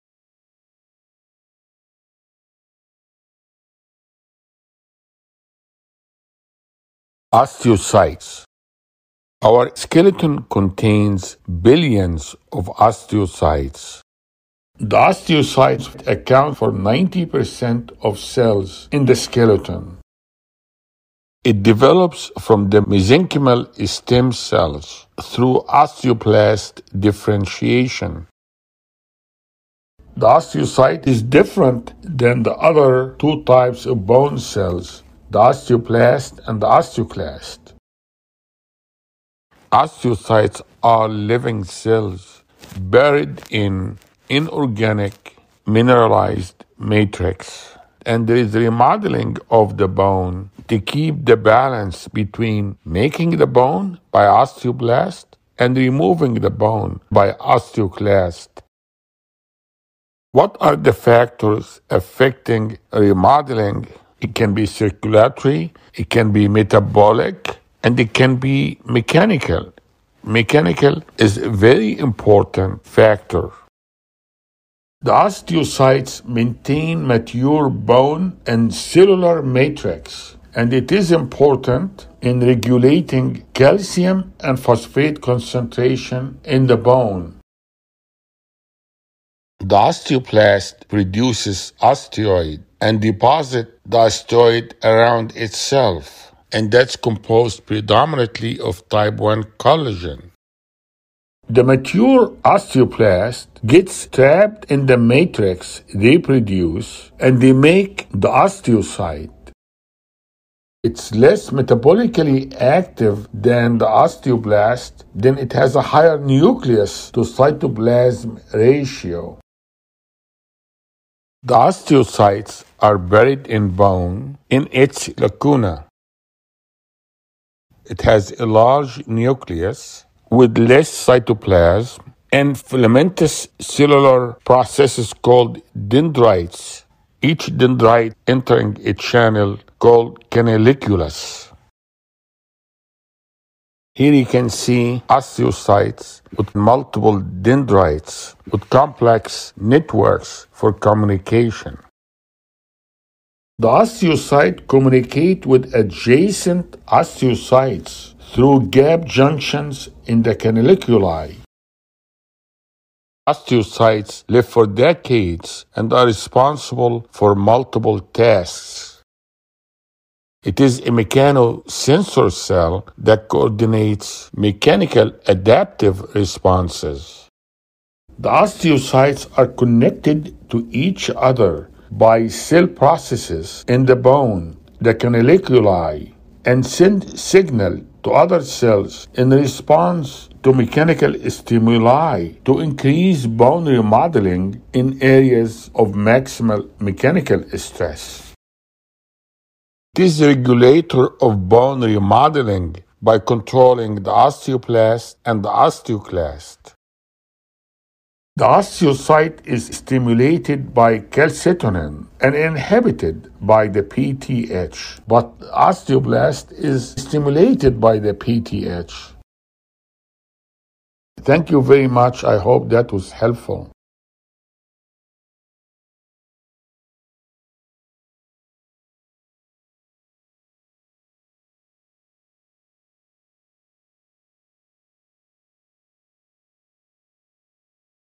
Osteocytes - Everything You Need To Know - Dr. Nabil Ebraheim
Dr. Ebraheim’s educational animated video describes osteocytes - bone remodeling cells.